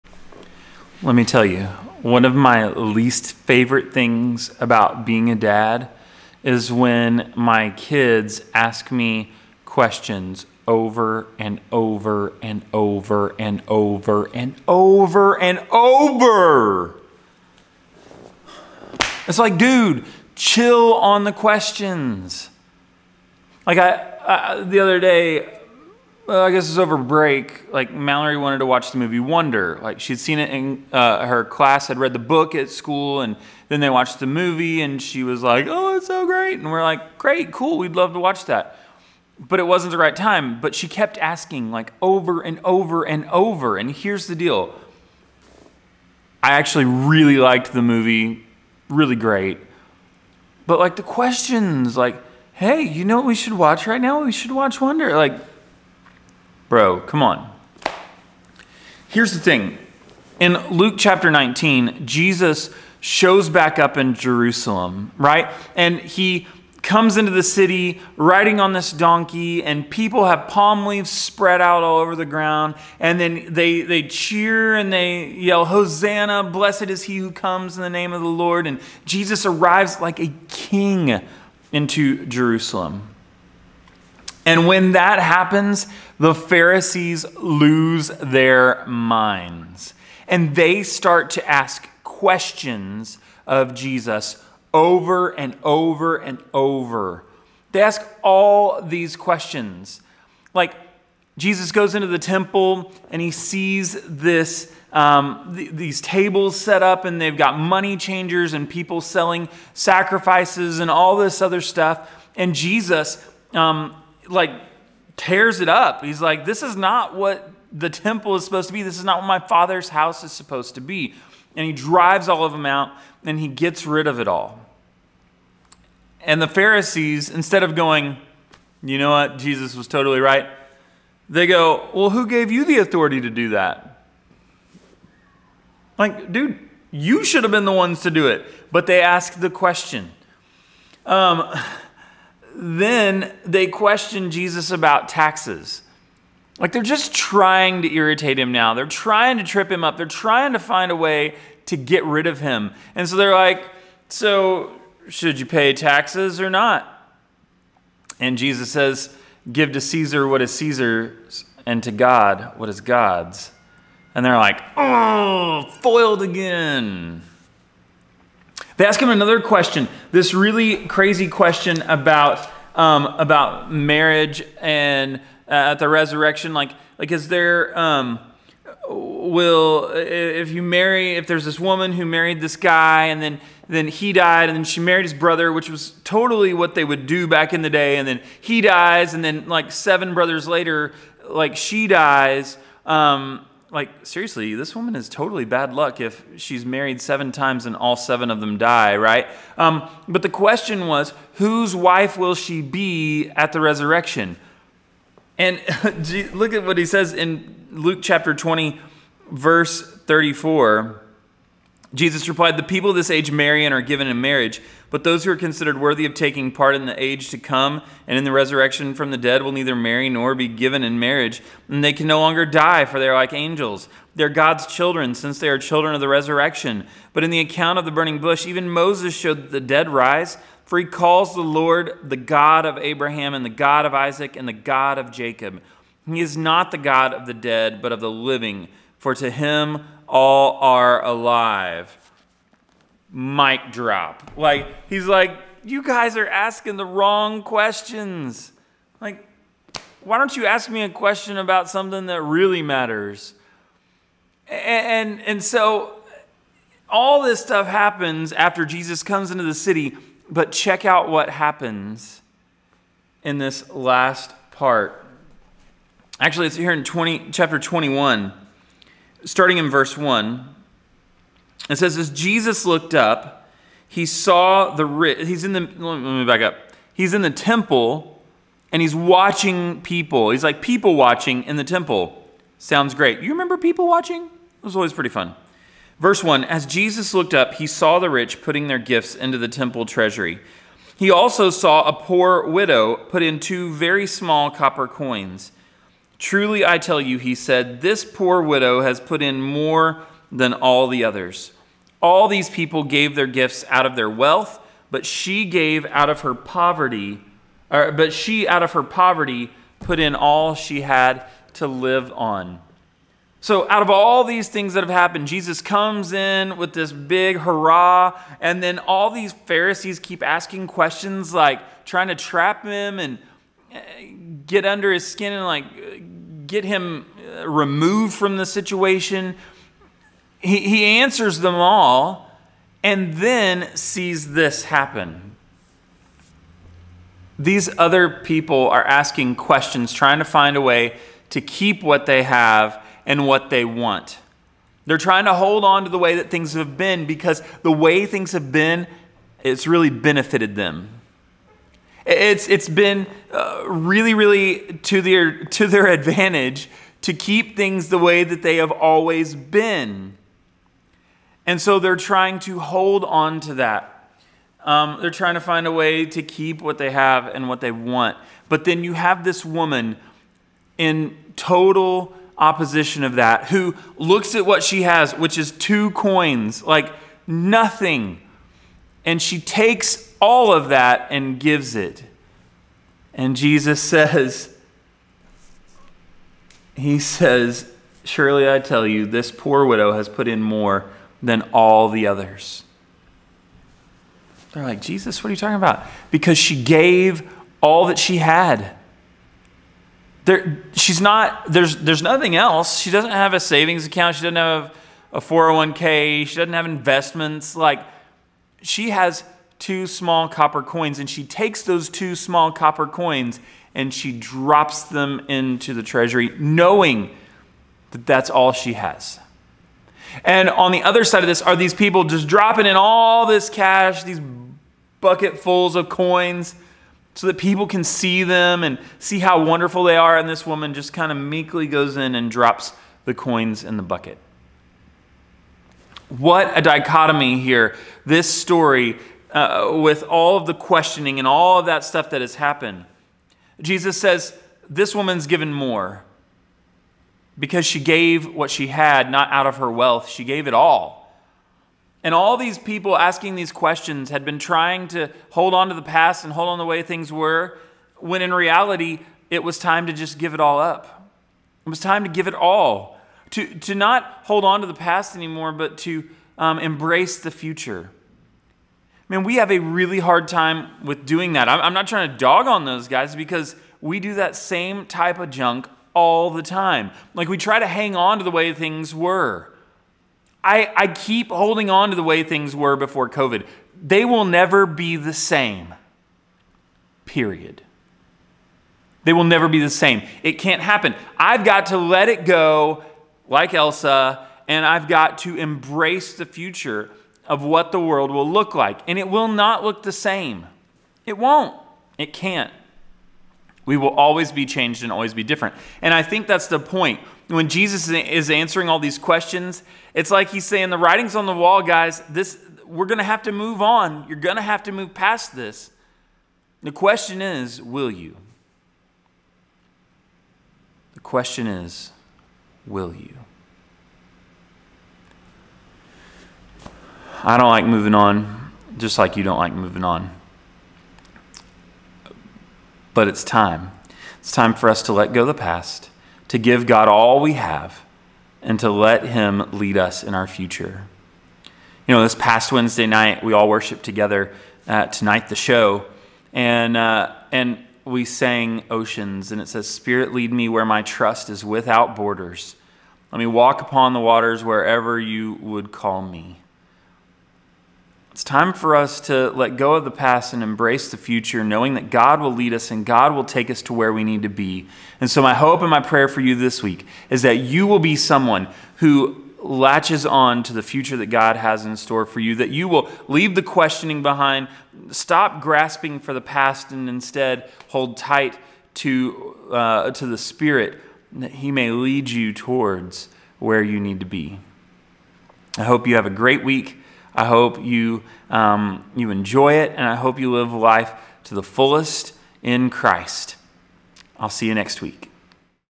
Questions and moving on - the Gospel of Luke wk 15
James talks about stories from Luke 19-21, questions, things that matter, and how we move on.